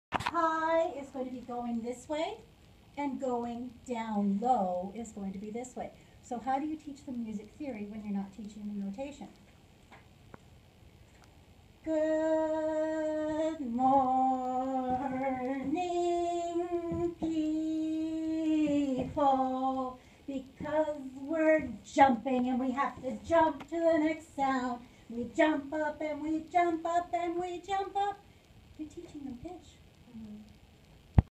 Special Needs how to teach pitch without notation
Shelagh McKibbon-U'Ren, Editor, Executive Director, Author and Examiner with Ultimate Music Theory - presented a workshop to Music Students at McGill University in Montreal. The question was - how can you teach pitch to special needs students when they are not learning about notation? Here is my answer.